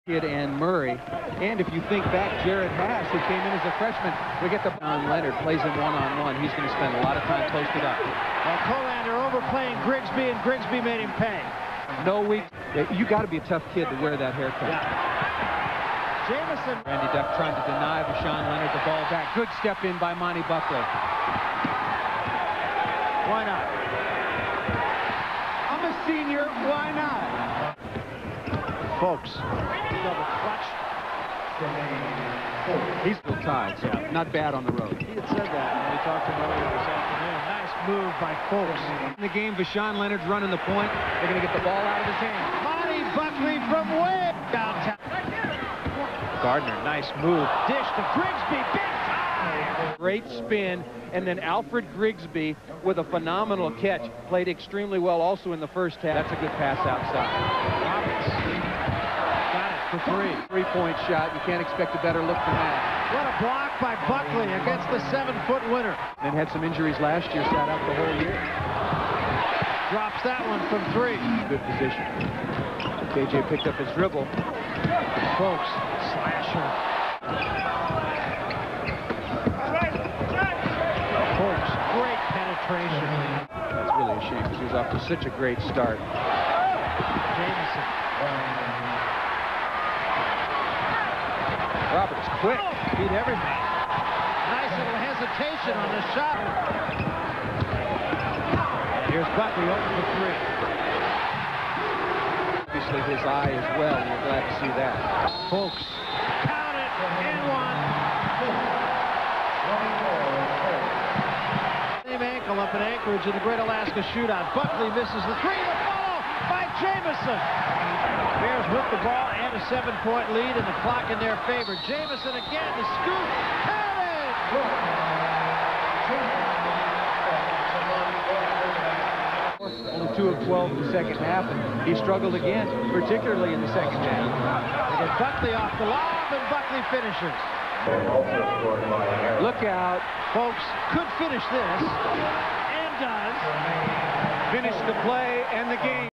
1994 Cal Basketball defeats Minnesota 82-75
Monty Buckley, Randy Duck, Tremaine Fowlkes, Jelani Gardner, Tony Gonzalez, Alfred Grigsby, Ryan Jamison, Sean Marks, Anwar McQueen, K. J. Roberts, Michael Stewart